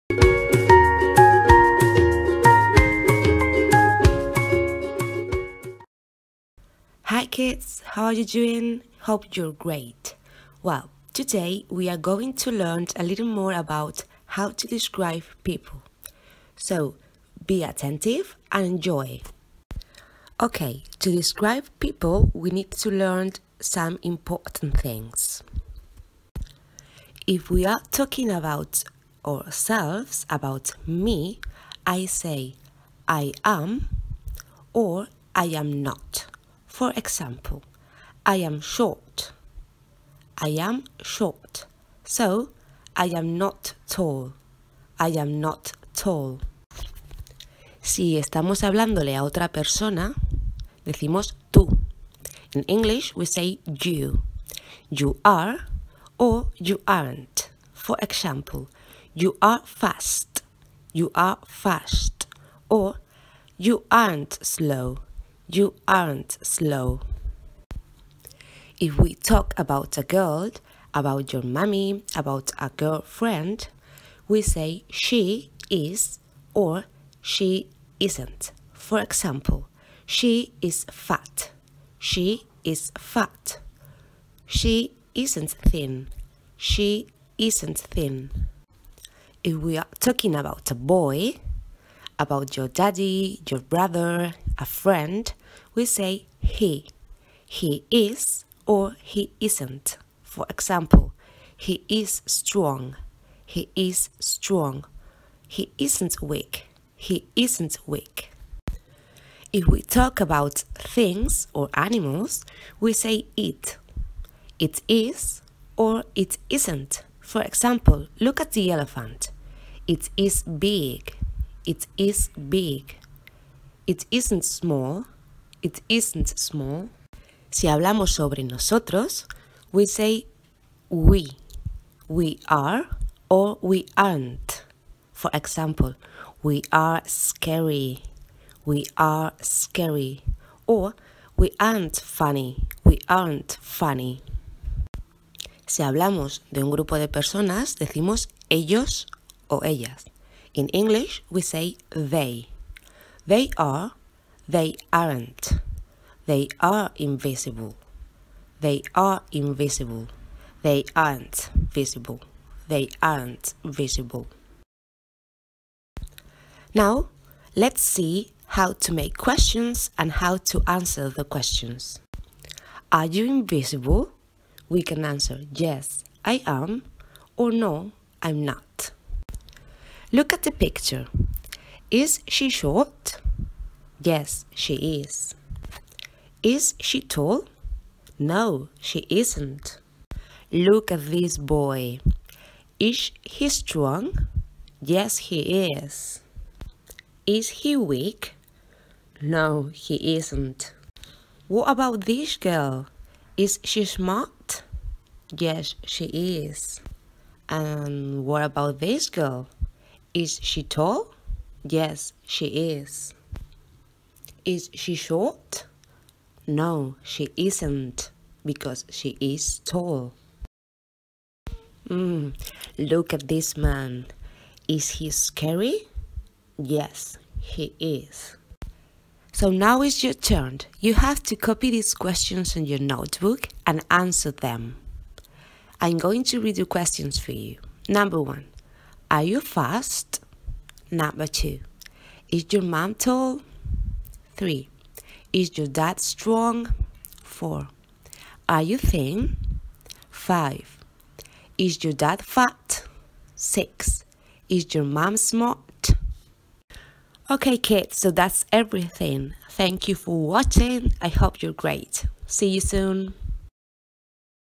DESCRIBING PEOPLE
Grammar video about how to describe people.
Verb "To be" in present simple.
2nd graders.